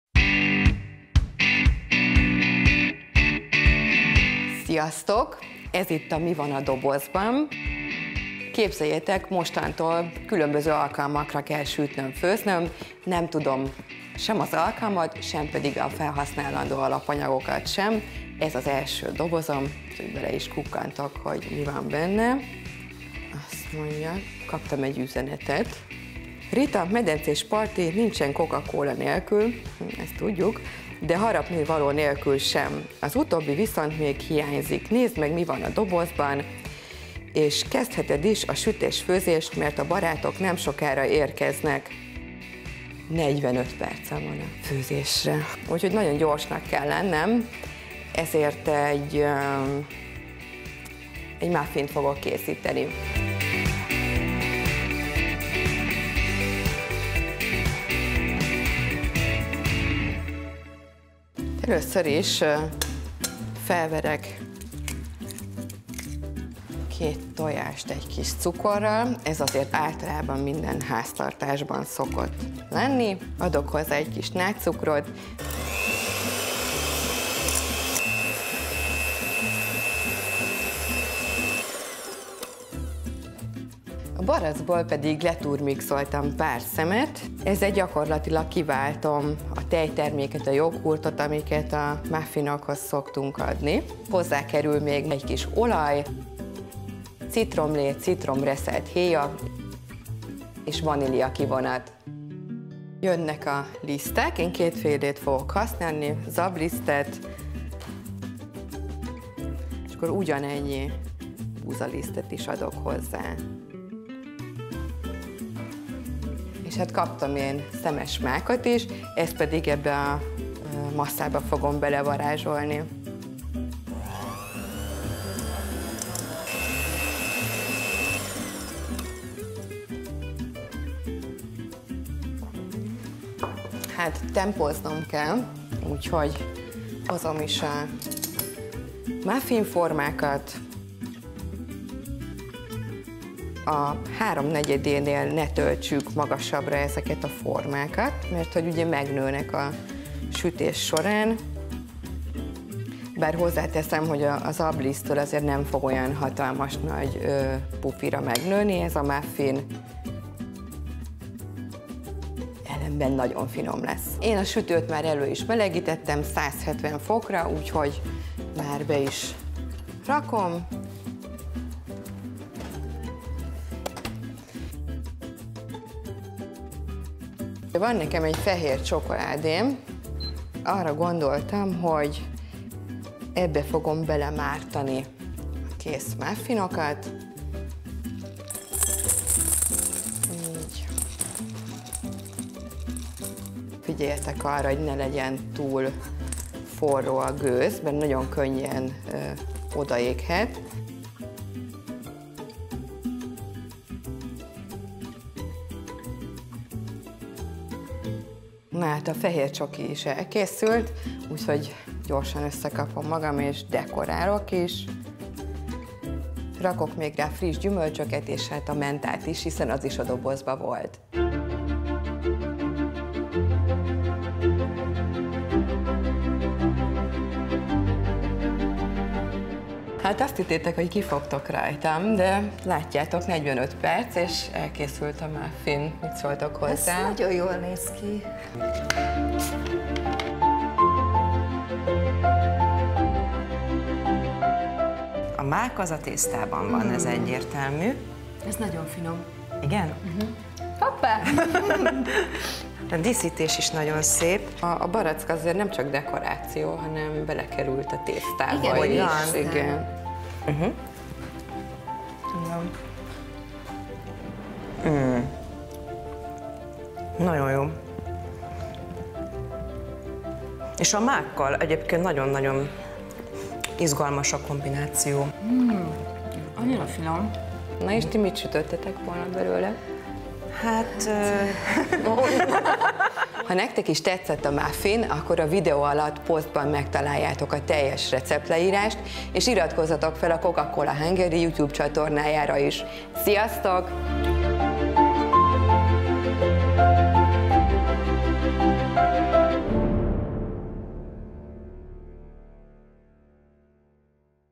Muffin medencés bulira -"Mi van a dobozban?" 1. rész Hadarik Ritával
Rita kapott 45 percet és 4 összetevőt, hogy csodát tegyen egy hirtelen felindulásból megszervezett  medencés partira. Nézd meg, hogy sikerült-e megugrania a "Mi van a dobozban?" 1. kihívást.

Hozzávalók 12 db muffinhoz:
10 dkg búzaliszt
10 dkg zabliszt
2 ek szemes mák
1 tk szódabikarbóna
10 dkg nádcukor
2 tojás
1,5 dl olaj (pl. kókuszolaj vagy sütő repceolaj)
2 dl turmixolt sárgabarack
1 db citrom leve és héja
kis vanília (kivonat)

A mázhoz:
10 dkg fehér csokoládé
+ gyümölcsök

Elkészítés:
1. A tojásokat és a cukrot fehéredésig keverjük.
2. A barackokat mossuk meg, majd teljesen pépesre turmixoljuk.
3. A cukros tojáshoz keverjük az olajat, a barack pépet, a citromlevet, a citrom reszelt héját, és a vaníliát.
4. A masszához hozzáadjuk a liszteket, a szódabikarbónát, és a szemes mákot is és alaposan elkeverjük.
5. Veszünk egy szilikonos muffin formát, vagy a hagyományos formát kibéleljük papír kapszlikkal, és mindegyiket kb. a háromnegyedéig töltjük a masszával.
6. Előmelegített (170 fokos) sütőben kb. 20-25 perc alatt készre sütjük őket.
7. A kihűlt muffinokat olvasztott fehércsokoládéba mártjuk, és tetszés szerint gyümölcsökkel, és a menta leveleivel dekoráljuk.

Már feliratkoztál a Youtube csatornánkra? Akkor kövesd a Coca-Cola Magyarországot további csatornáinkon is!